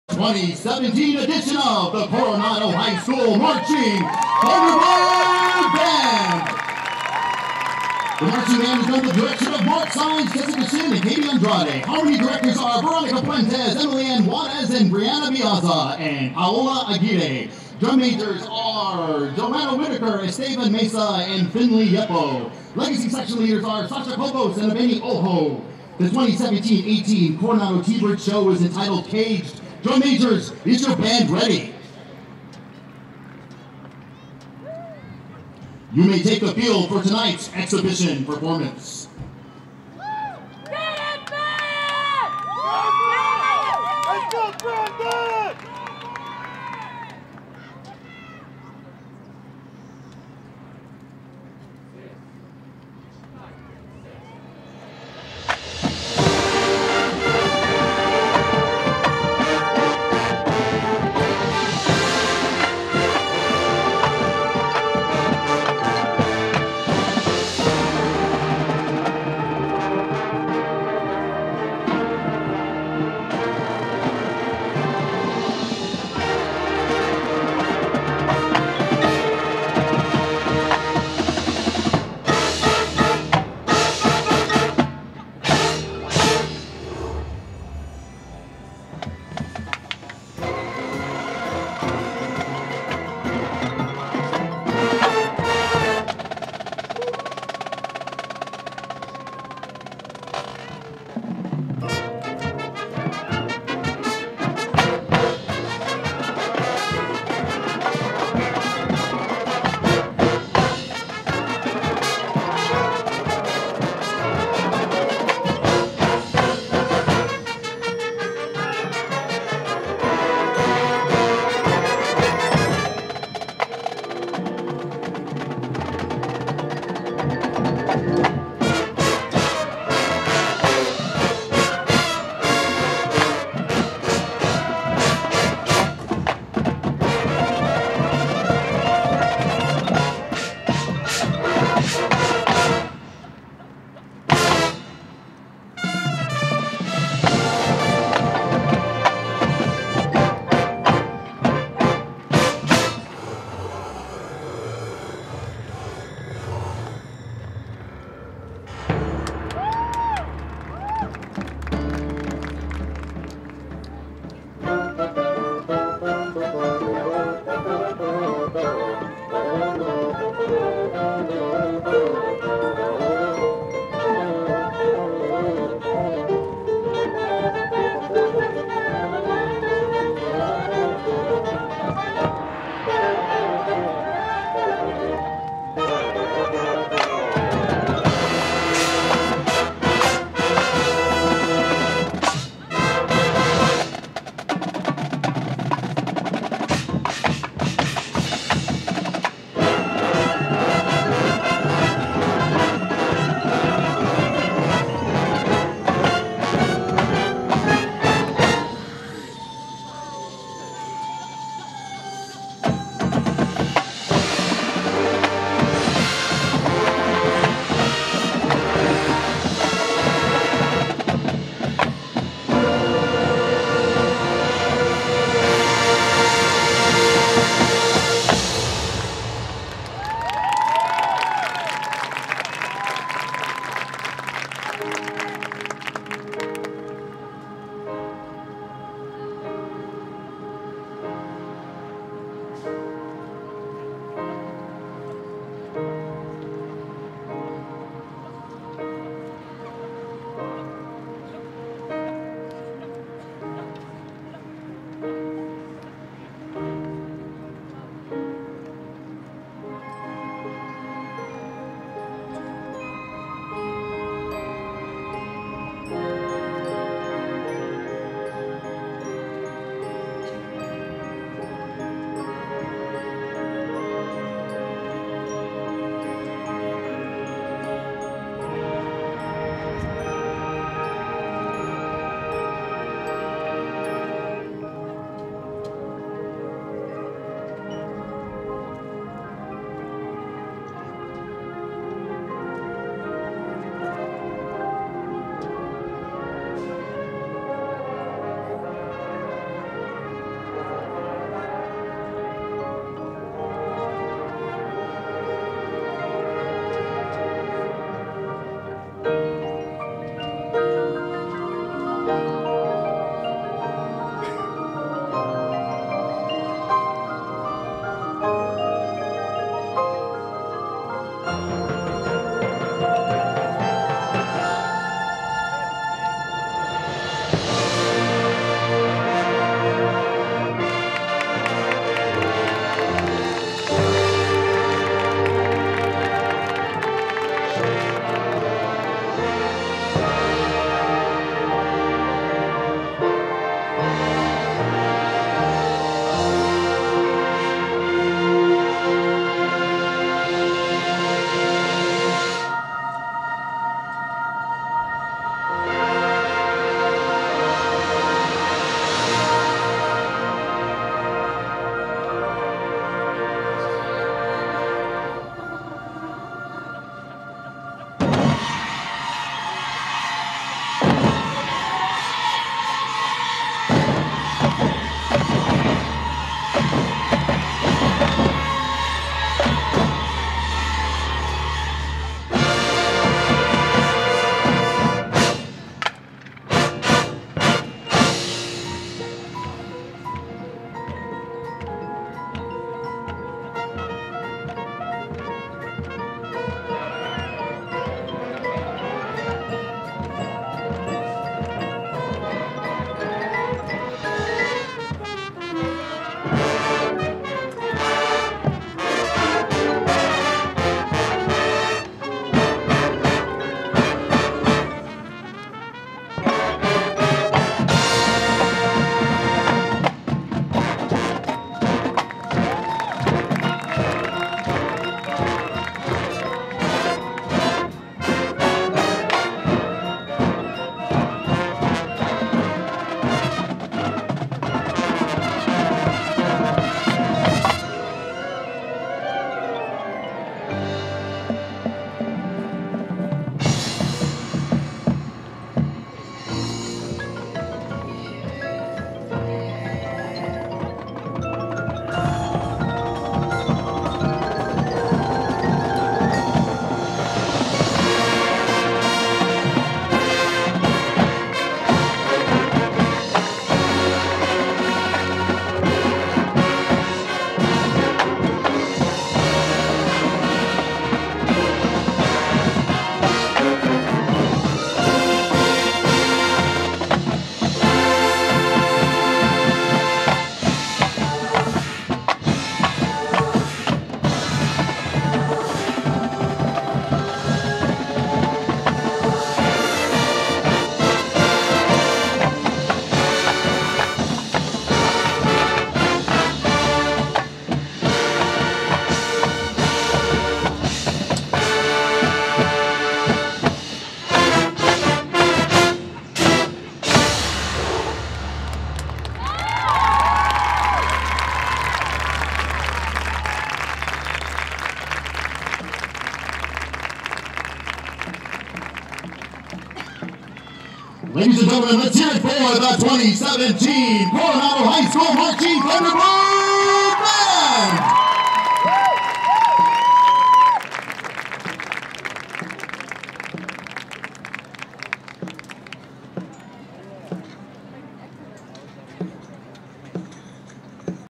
CORONADO HIGH MARCHING BAND PERFORMANCE COLOR GUARD T-BIRD T BIRD INVITATIONAL 2017
Great Job!